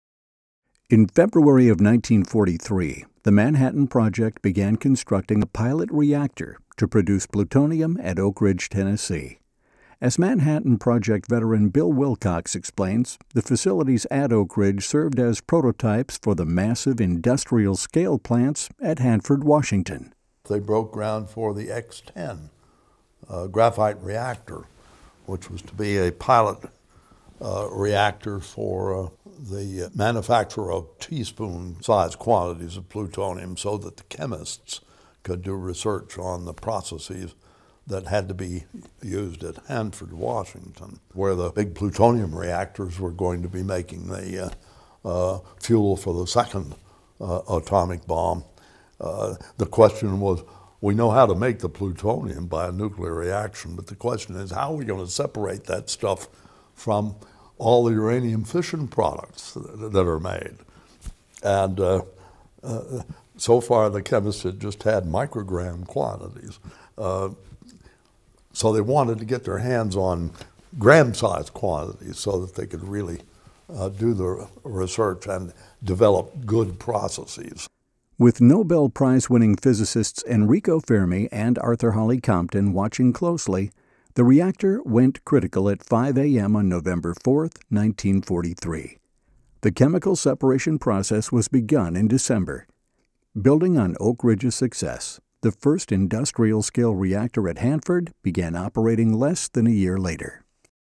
The Pilot Reactor
Manhattan Project veteran and Oak Ridge City Historian Bill Wilcox describes why the X-10 Graphite Reactor was built.